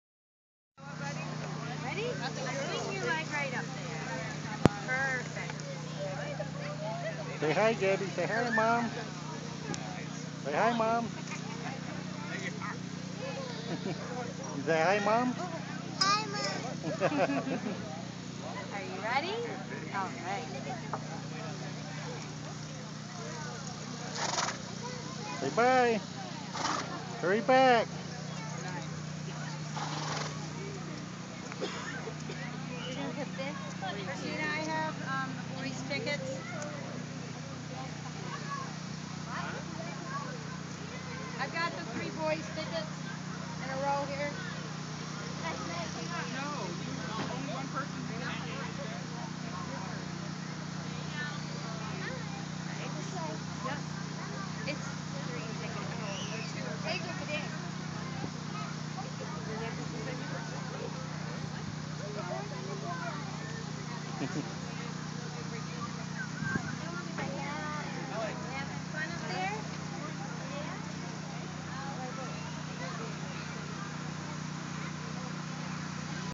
Gabby Riding
Gabby was caught riding this horse after picking apples at Behilings in Mexico, N.Y.